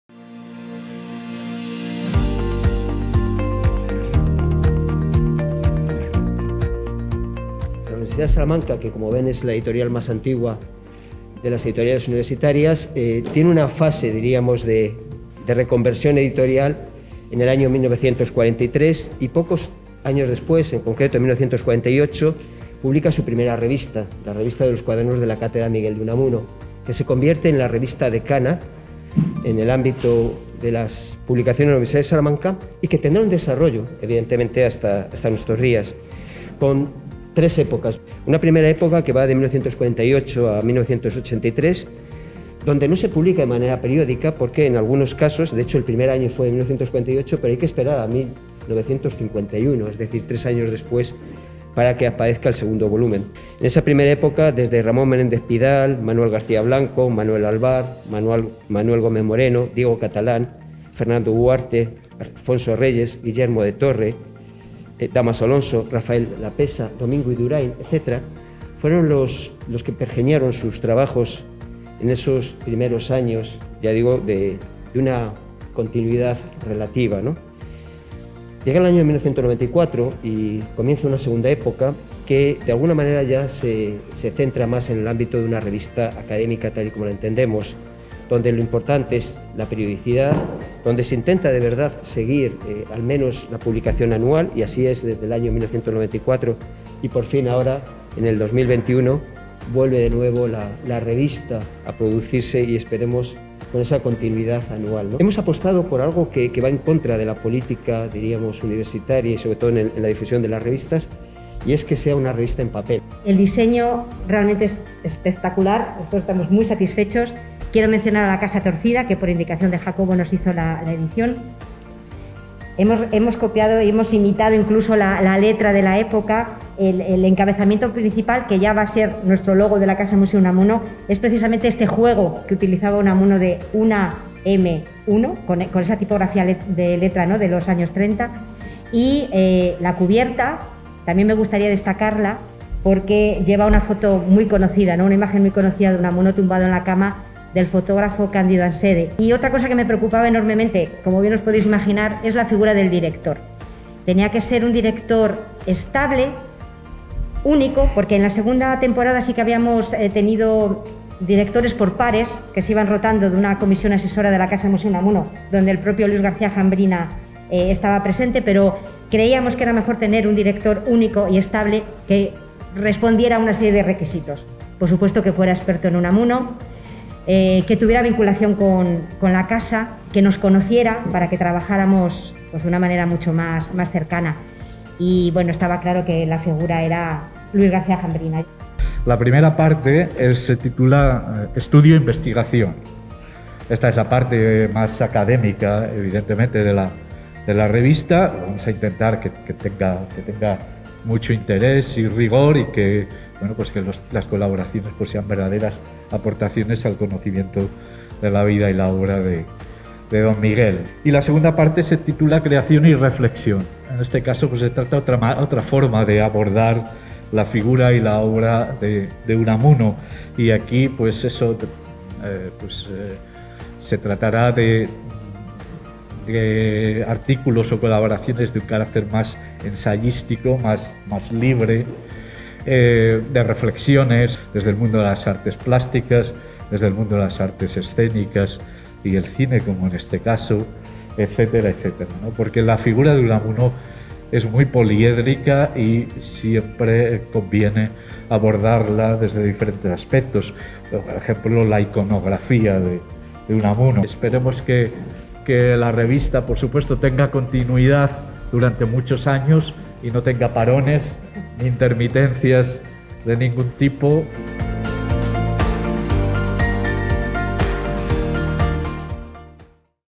"Los Cuadernos de la Cátedra Miguel de Unamuno"
Ediciones Universidad de Salamanca presenta el último número de "Los Cuadernos de la Cátedra Miguel de Unamuno". Se inicia la tercera etapa de esta revista con el monográfico "La palabra frente al autoritarismo".